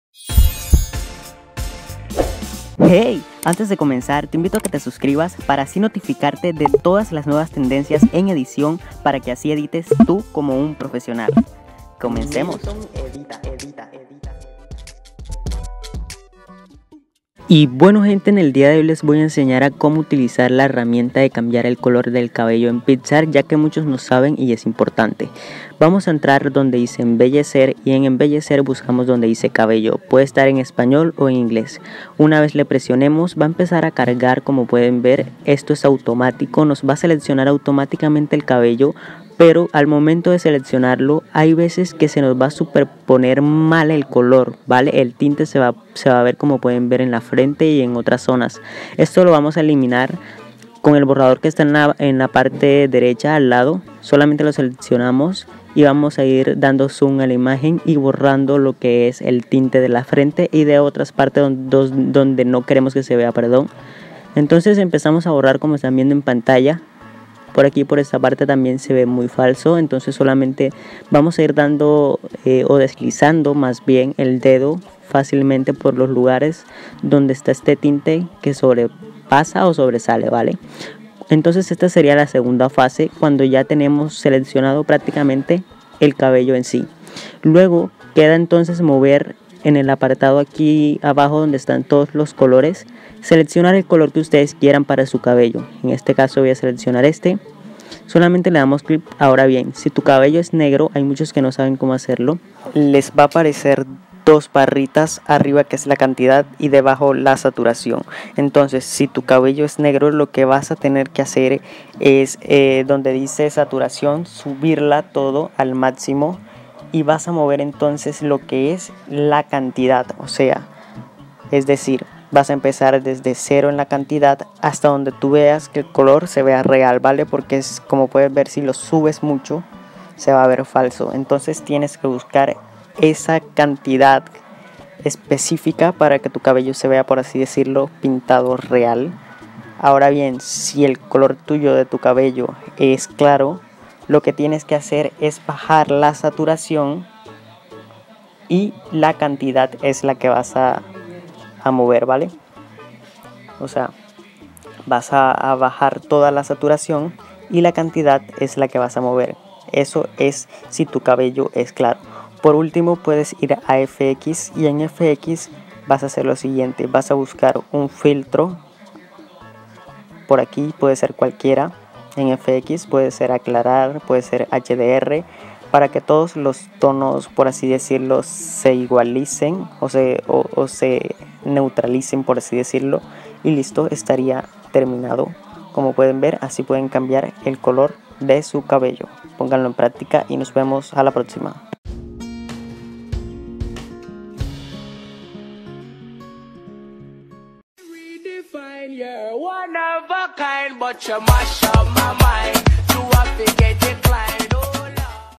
cómo CAMBIAR el COLOR de TU PELO usando PICSART | TUTORIAL | fácil )
Nuevo Tutorial  en picsart cambiar color de pelo
      ¡HOLA AMIGOS!
🔻NUEVO VIDEO DE EDICIÓN DE FOTOS¡
🔻¿QUE EFECTO QUIERES QUE HAGA?
🔻❤️LOS AMOOO!!❤️🔥